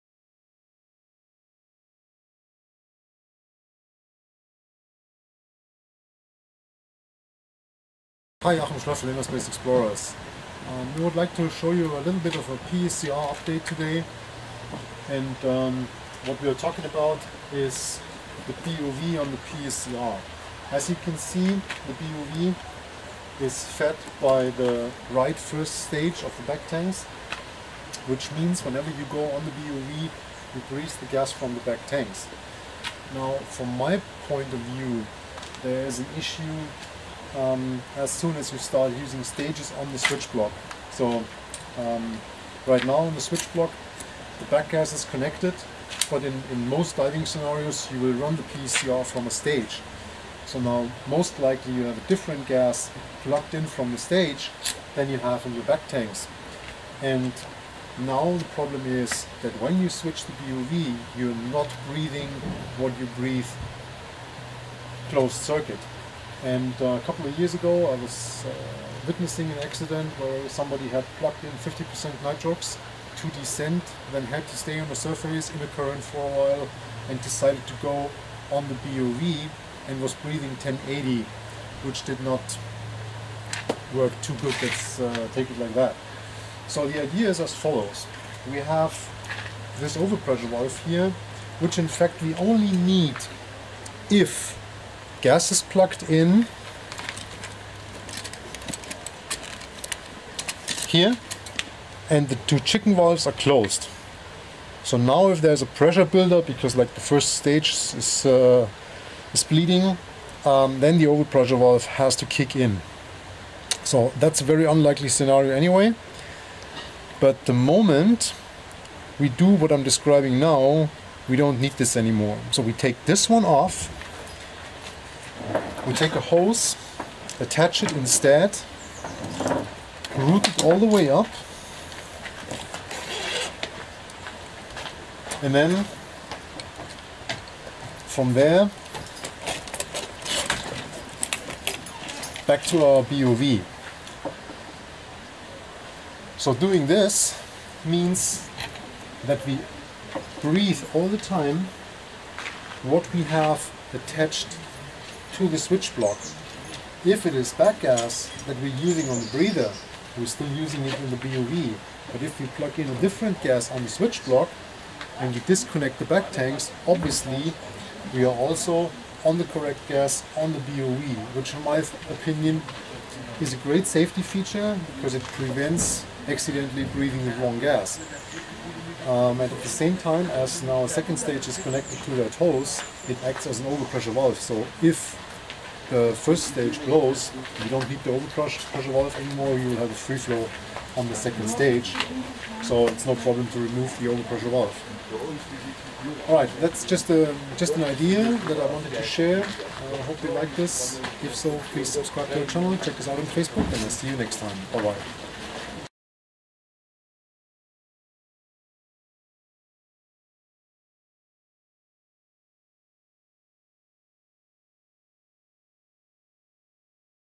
How to improve the Switchblock of a pSCR Rebreather
an idea of how to make sure to breath the right gas when you switch to your BOV all the time.